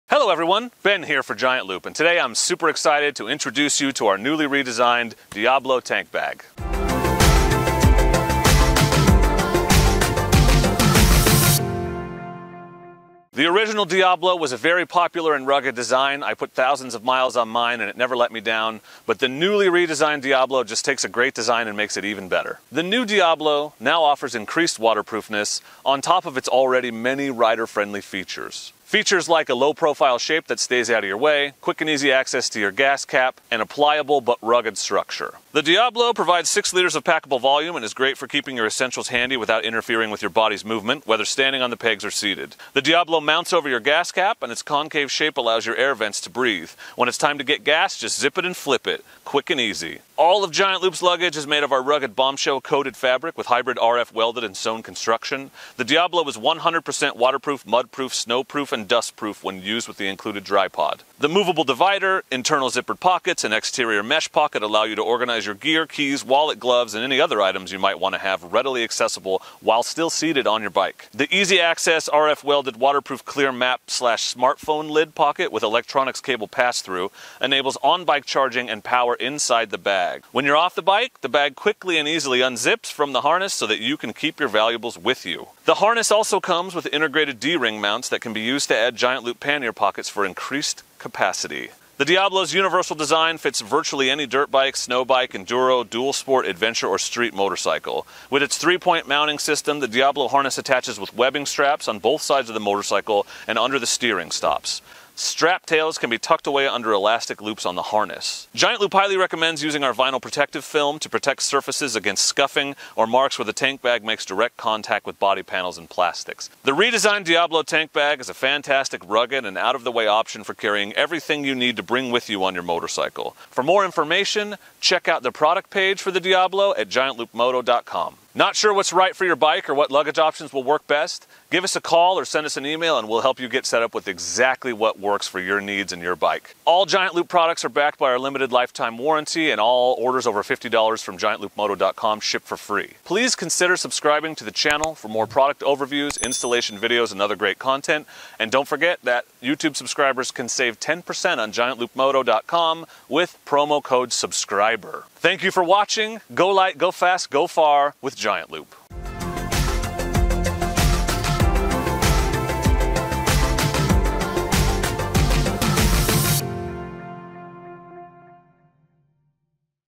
The New Giant Loop Diablo Tank Bag: Perfect for Your Dirt, Snow, Adventure, or Dual Sport Motorcycle
The newly-redesigned Giant Loop Diablo Tank Bag takes what was quite possibly the best dual sport tank bag and one of the best tank bags for adventure bikes and makes it even better.  Designed to work with virtually any motorcycle (dirt, street, enduro, adventure, dual sport, and beyond), the Diablo is one of the best on or off road motorcycle tank bags you'll find on the market today.

More info on the Diablo Tank Bag can be found

The new Diablo now offers increased waterproofness along with its many rider-friendly features like a low profile shape that stays out of the way, a pliable but rugged structure, and quick, easy access to your gas tank.  The Diablo provides six liters of packable volume and is great for keeping your essentials handy without interfering with your body’s movement, whether standing on the pegs or seated.

The Diablo mounts over your gas cap and its concave shape allows your air vents to breathe.  When it’s time to get gas, just zip it and flip it.  Quick and easy.  All of Giant Loop’s luggage is made of our rugged Bomb Shell coated fabric with hybrid RF welded and sewn construction.  The Diablo is 100% waterproof, mud proof, snow proof, and dustproof when used with the included Dry Pod.

The movable divider, internal zippered pockets, and exterior mesh pocket allow you to organize your gear, keys, wallet, gloves, and any other items you might want to have readily-accessible while seated on your bike. The easy-access, RF-welded waterproof clear map/smartphone lid pocket with electronics cable pass through enables on-bike charging and power inside the bag.

When you’re off the bike, the bag quickly and easily unzips for the harness so you can keep your valuables with you.  The harness also comes with integrated D-Ring mounts that can be used to add Giant Loop Pannier Pockets for expanded capacity.

The Diablo’s universal design fits virtually any dirt bike, snow bike, enduro, dual sport, adventure, or street motorcycle.  With its three-point mounting system, the Diablo harness attaches with webbing straps on both sides of the motorcycle and under the steering stops. Strap tails can be tucked away under elastic loops on the harness.

Giant Loop highly recommends using our Vinyl Protective Film to protect surfaces against scuffing or marks where the tank bag makes direct contact with body panels and plastics.

The redesigned Diablo tank bag is a fantastic, rugged, and out of the way option for carrying everything you need to bring with you on your motorcycle.

(excluding Gas Bags, sale priced items, gift certificates and rides/events)